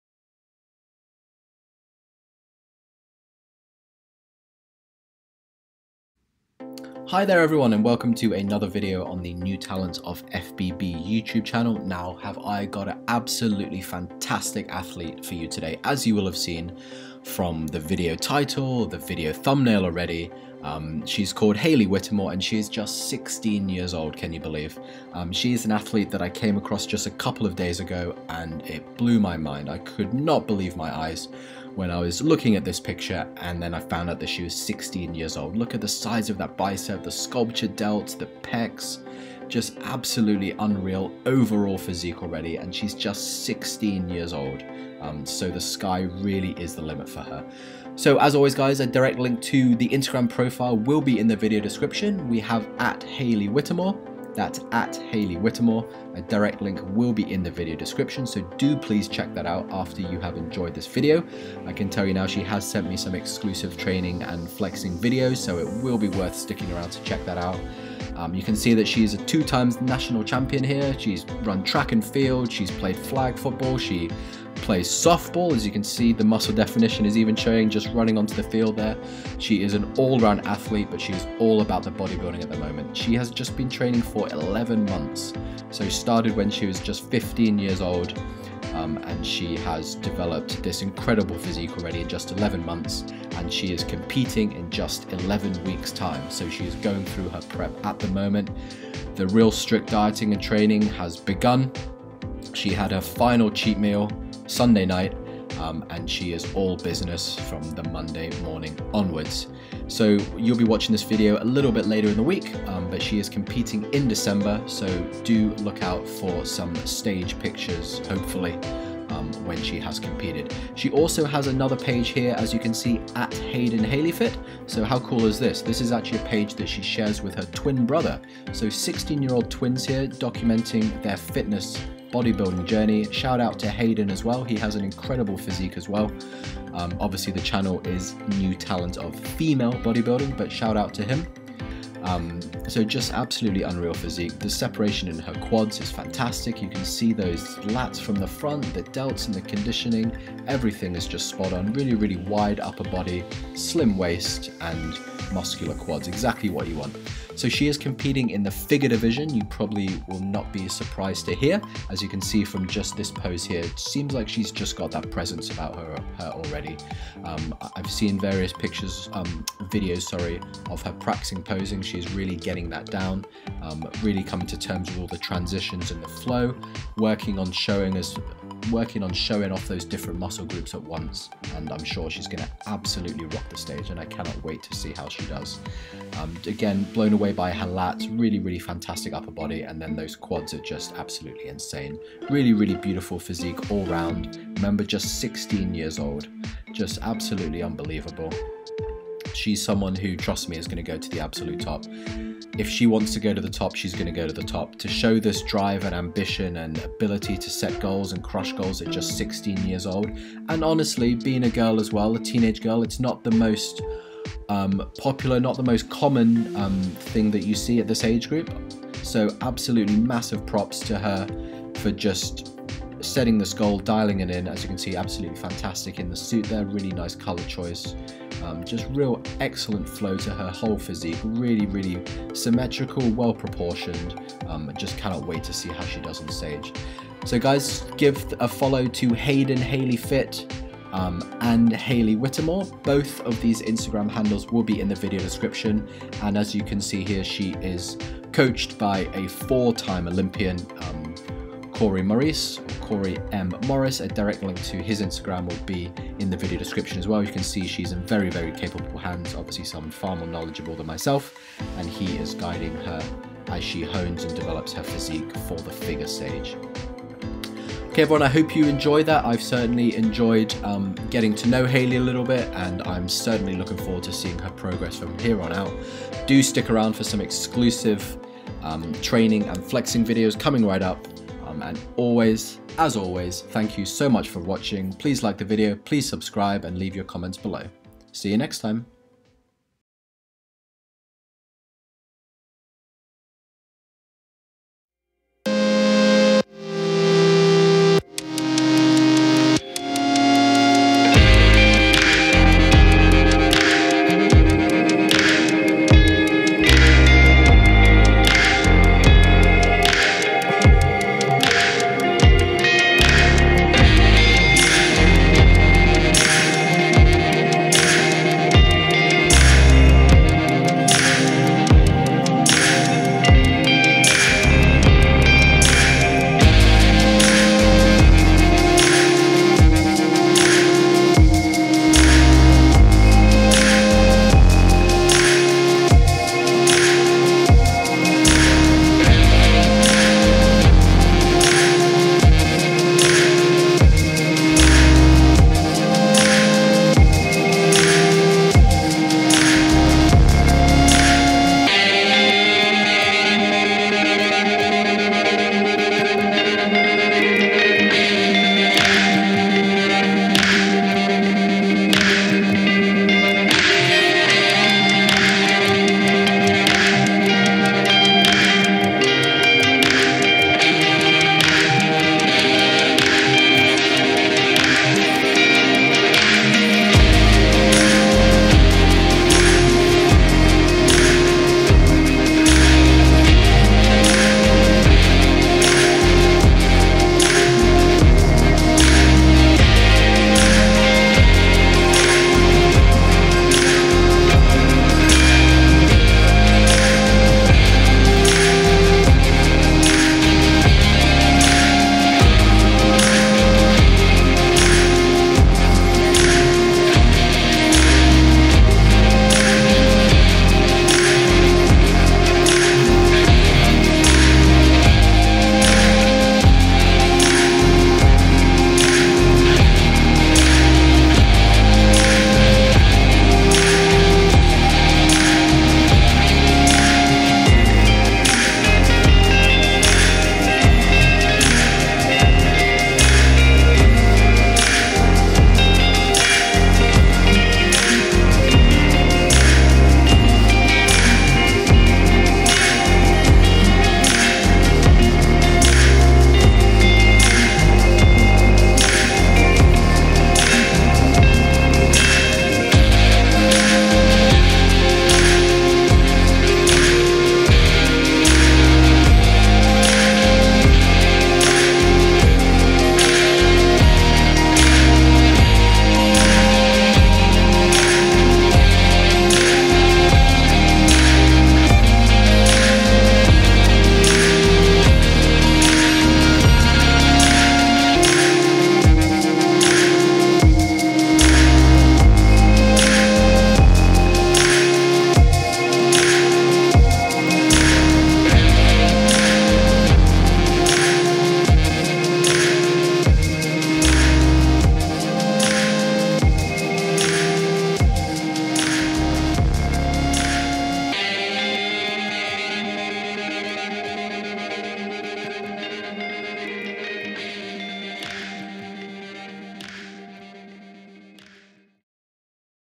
Haylee Whittemore || 16 years old || Figure Competitor || USA
New talents of FBB: Haylee Whittemore, 16 year old figure competitor from the USA!